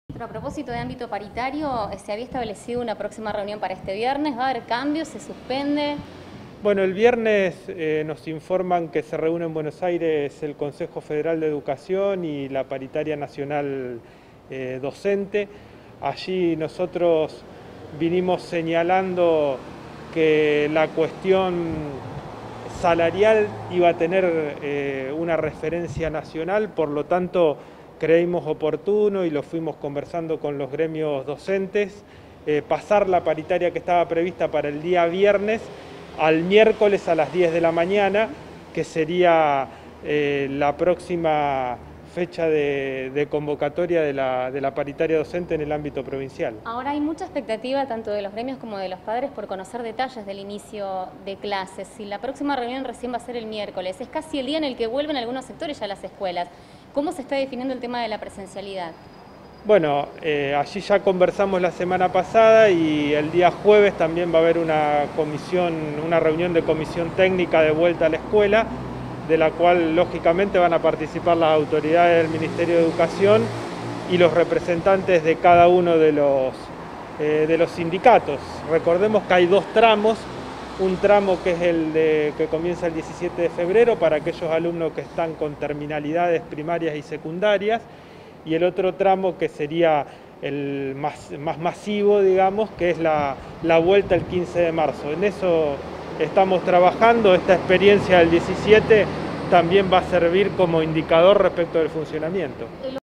LA NUEVA REUNIÓN DE LA PARITARIA DOCENTE SE CONCRETARÁ EL MIÉRCOLES 17 DE FEBRERO
JUAN PUSINERI – MINISTRO TRABAJO